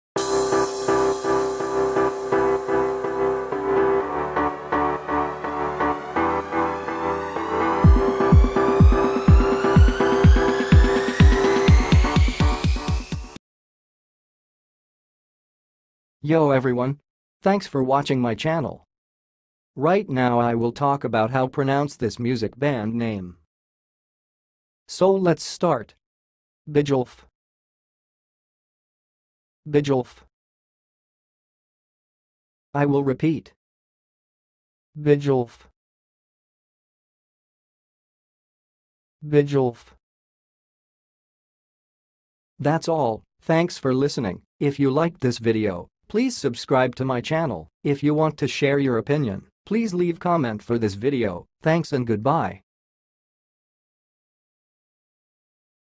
BIGELF - HOW TO PRONOUNCE IT!? (HIGH QUALITY VOICE)
Watch and listen how to say and pronounce Bigelf music band name!
Compare your pronunciation with our video!